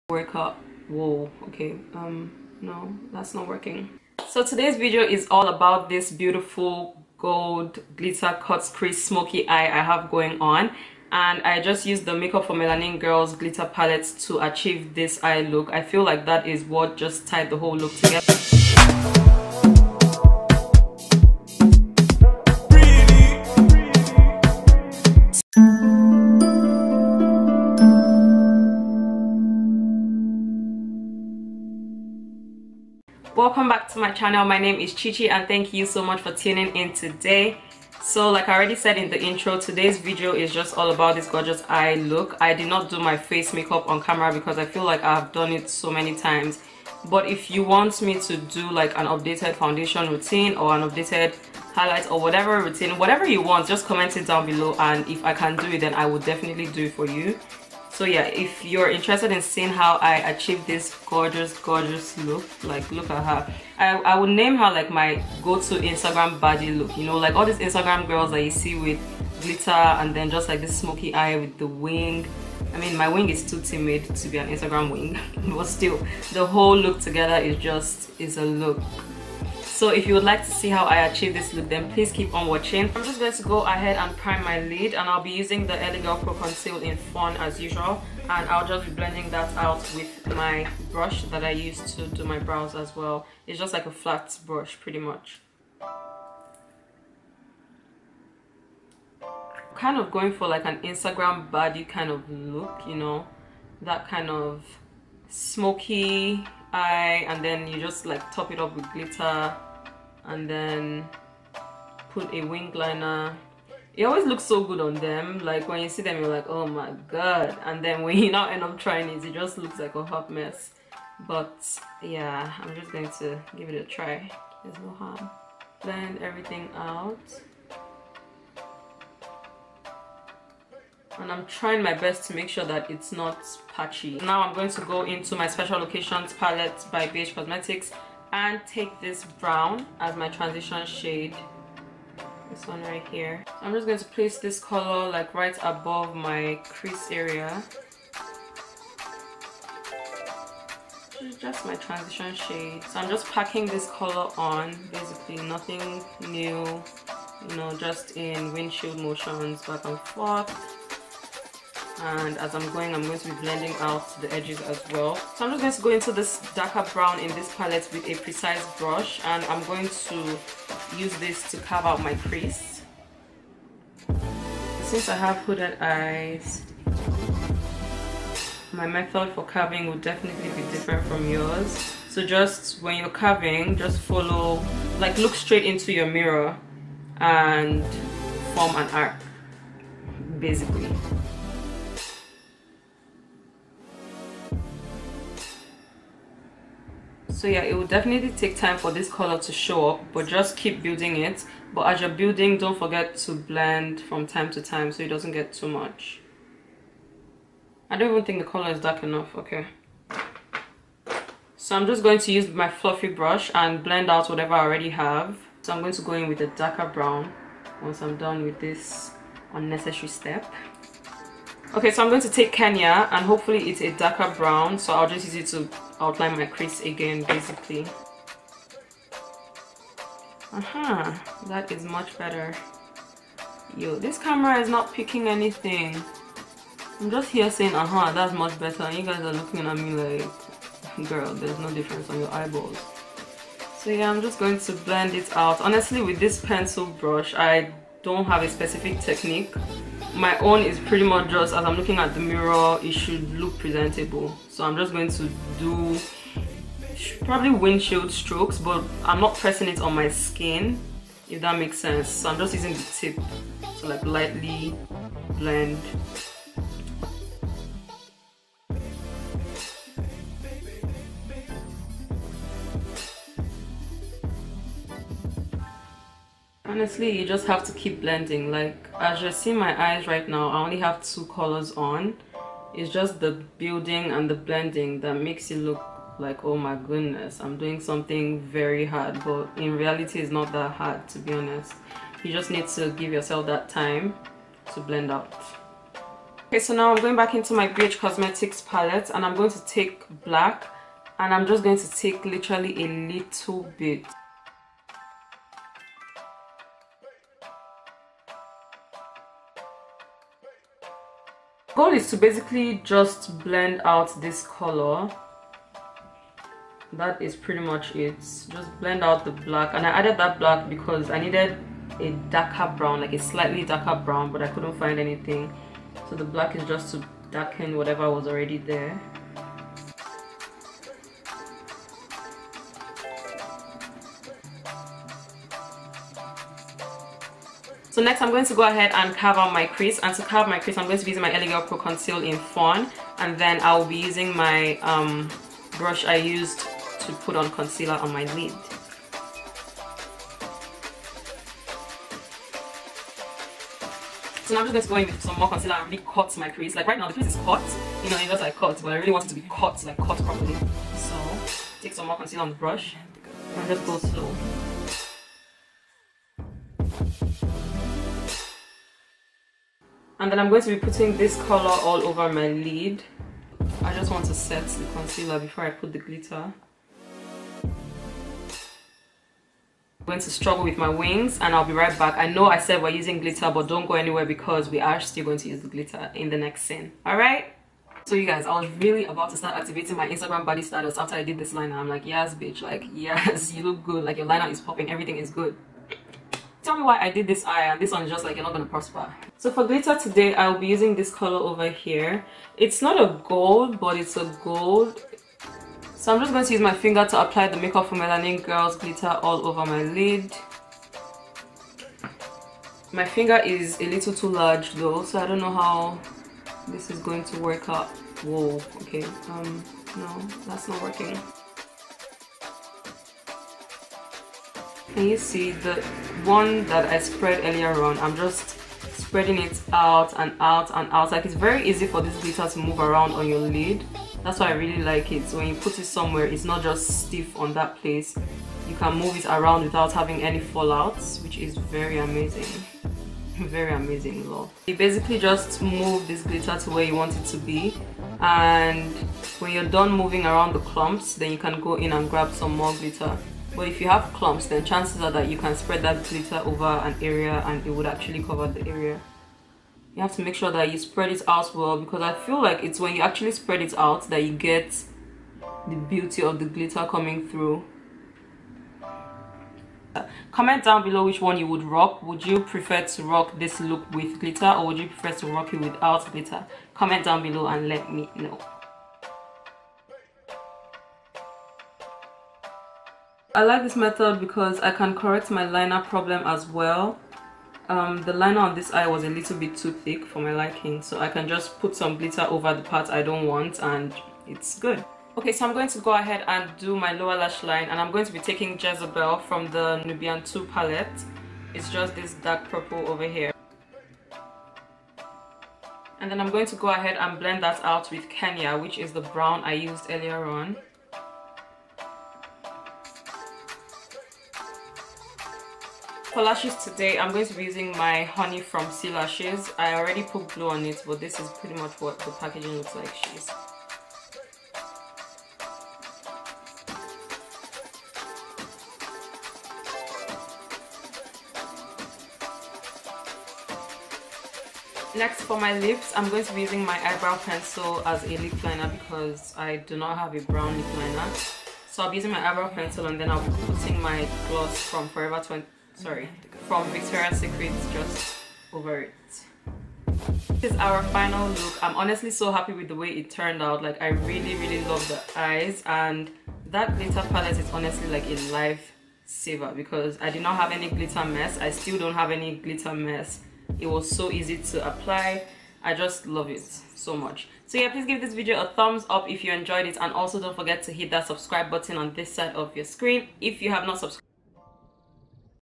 GOLD GLITTER CUT CREASE ft MFMG Glitter Palette |Chinyere I.
Hey Babies! 😍 This Gold Glitter cut crease left me feeling like and instagram baddie so i decided to share so that we can be baddies togetherrr. I used the makeup for melanin girls (mfmg) glitter palette. Did i like it? Go ahead and watch the video for an answer